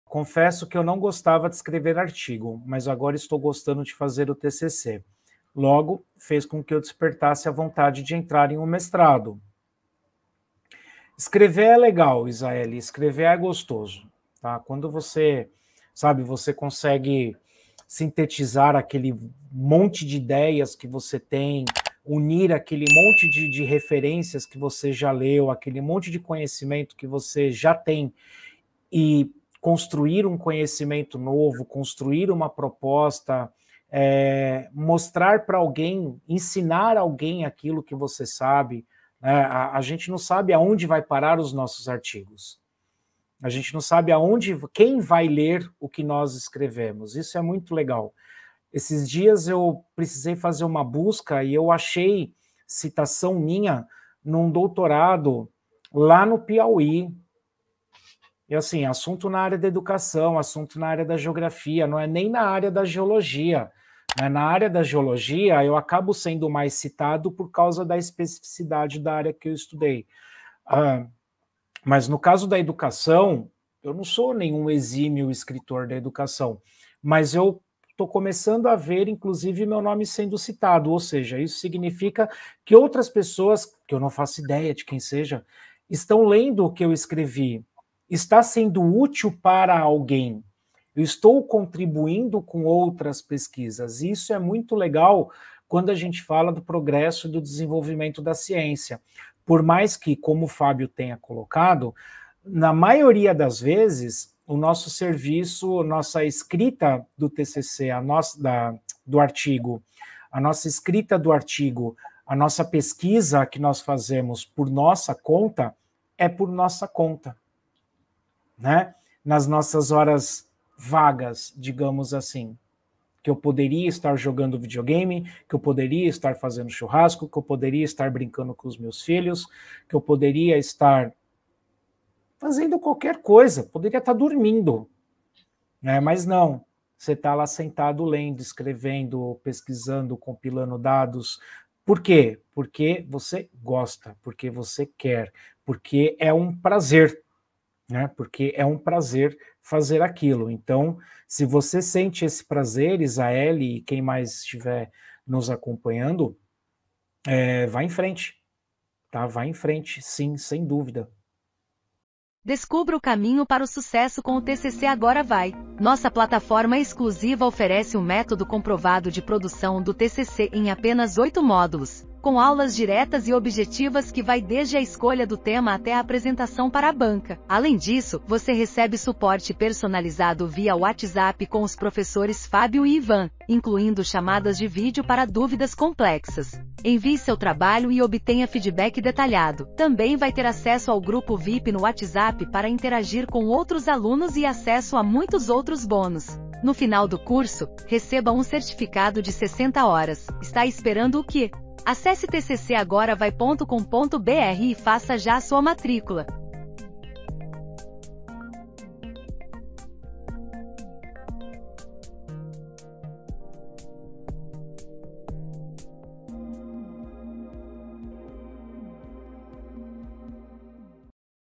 Você gosta de escrever?
E você? gosta de escrever? Este vídeo aborda a experiência transformadora de escrever um TCC e como isso pode despertar o interesse em prosseguir com estudos de mestrado. Vamos compartilhar nossa jornada e destacar  como a escrita acadêmica se tornou uma atividade prazerosa e significativa, permitindo a síntese de ideias e a construção de novos conhecimentos. Também tratamos da satisfação de ver seu trabalho sendo citado em outras pesquisas, contribuindo para o avanço da ciência.

Tem dúvidas? Chama no whatsapp